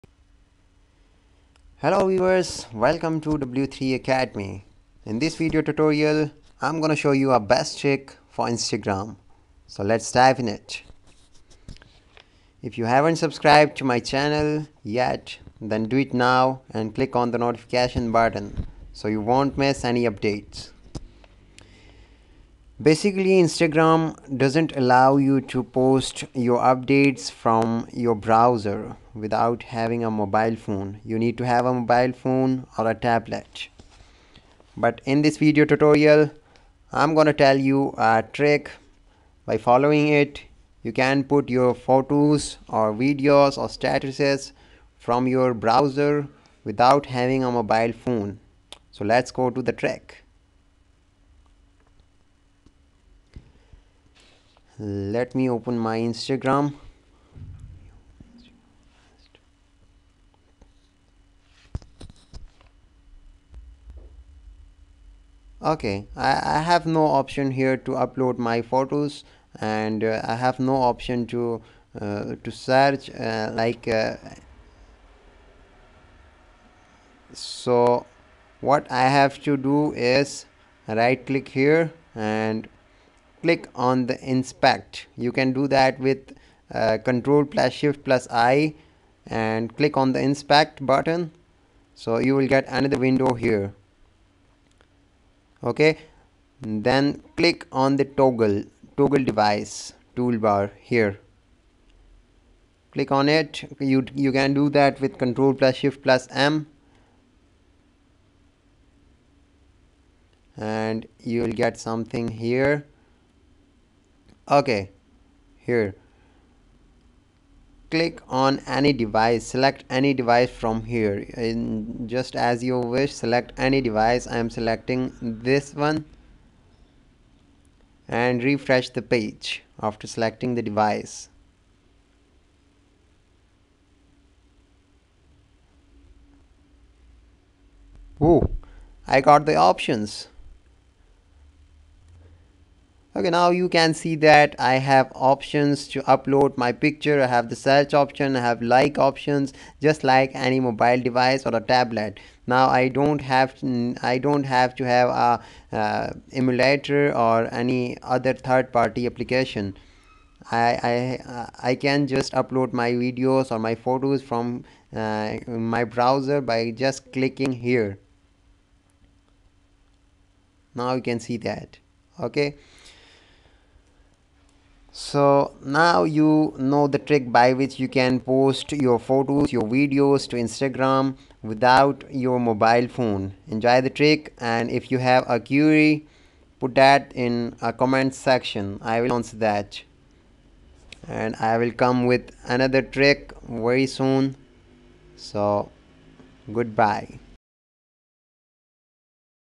how to post on instagram from your PC (2019 Working)
In this video, you will get the best and very simple trick to post on instagram from PC directly. Alot of people were asking me questions like " Can I post on instagram from my comput
er, or can you post on instagram from mac", so I am here with the solution of posting on instagram from PC. You can use instagram on your PC, just like your mobile, without any emulator or any 3rd party application.
Here I wanna clear one thing, you can use instagram on mac as well as on windows by using this trick.

Instagram usually doesn't allow to post your post from your PC, but with this simple trick, you can post on instagram from your PC for free without any emulator or 3rd party website.

W3Academy is the emerging channel on YouTube aims to provide the best solution to your problems related to technology.